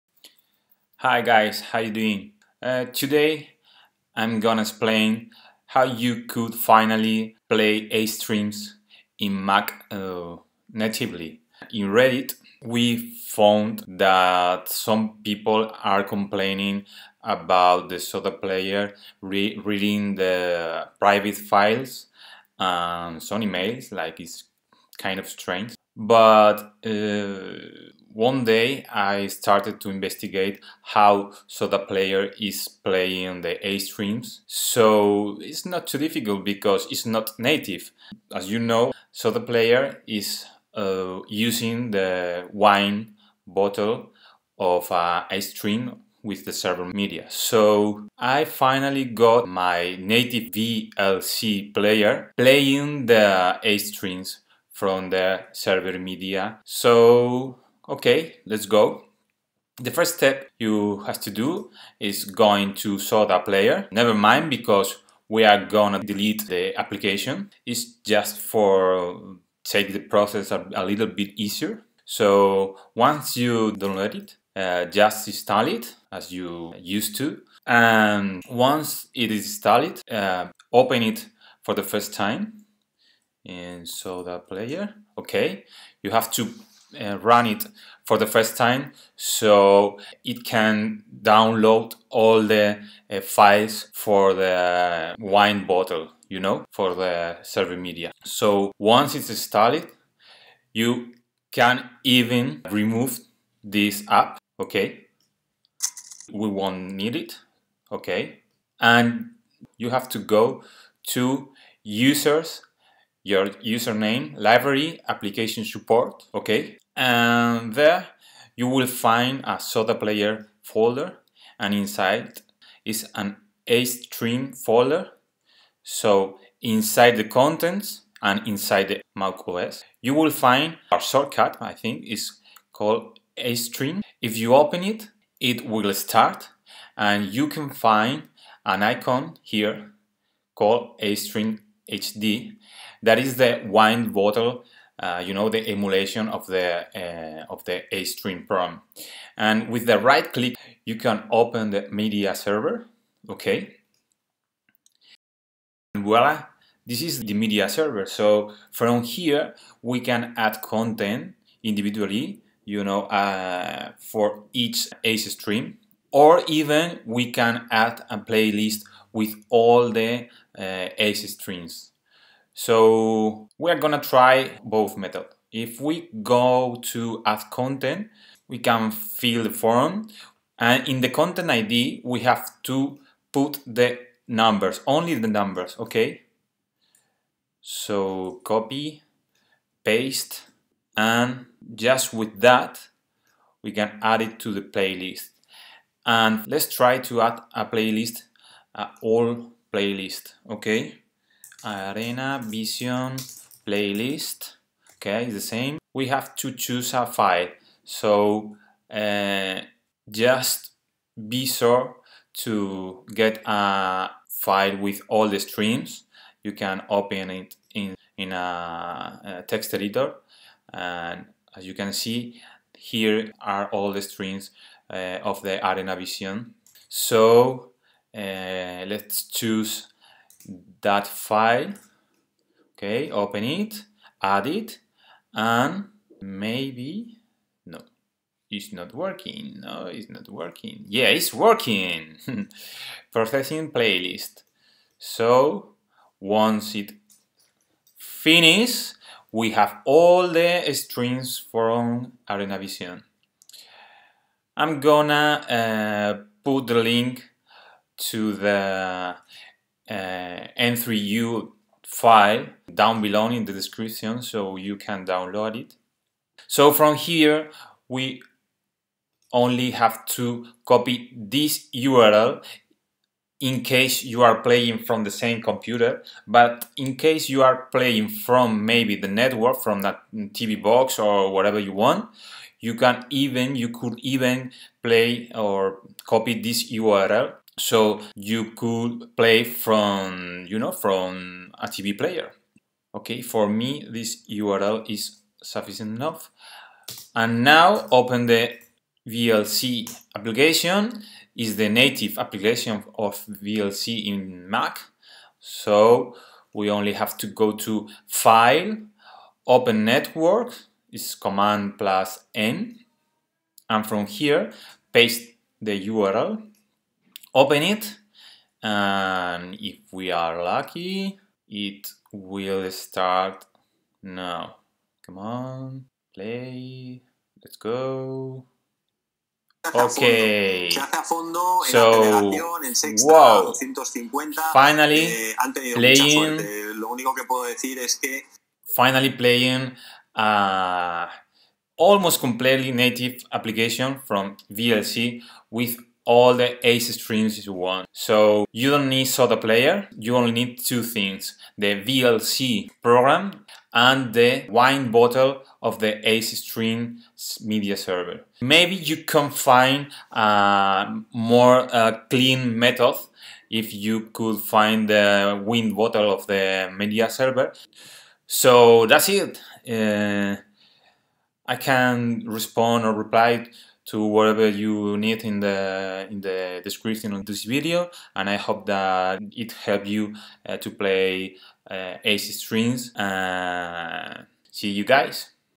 How to play acestream in Mac OS with VLC natively
In this video I will show you how to play acestream streams natively in Mac OS. We will use the Acestream Media Server and the native VLC app for Mac.

Here are the steps:
-Download Soda Player (we need to easely download the Acestream Media Server)
-Install it and run it
-IMPORTANT STEP! Try to play an acestream at least one time to Force Sodaplayer to download all the needed Wine files (sorry I forgot to mention this in the video)
-Delete the SodaPayer.app (we won’t need it, but just delete the app)
-Go to /Users/username/Library/Application Support/Soda Player/acestream/Contents/MacOS and run Acestream shortcut
-Right click in the Acestream Icon and open the Media Server
-Add some acestream content to the mediaserver using the “Add content” or “Add playlist”. For the acestream only use the numbers without the protocol of the url. (more info in the video)
-Grab the Media Server url from the “Playlist URL” and copy it
-Open the VLC.app and open a Network stream with Cmd+N
-Paste the Media Server Playlist url and…

M3U file for acestreams of Areanvision (maybe could be outdated in the future)